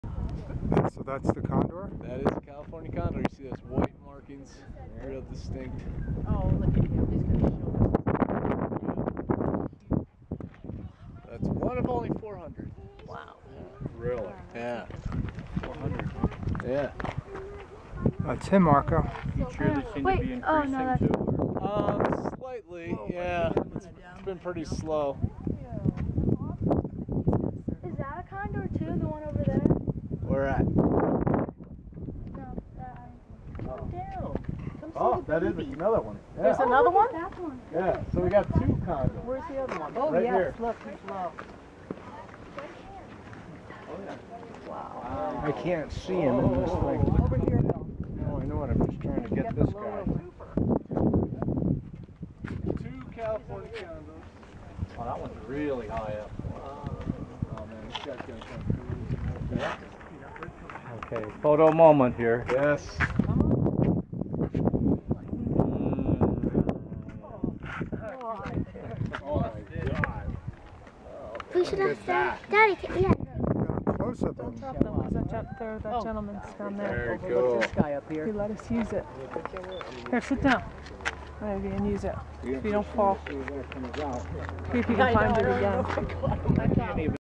California Condors Flying Over Grand Canyon
The critically endangered California Condor flying over the grand Canyon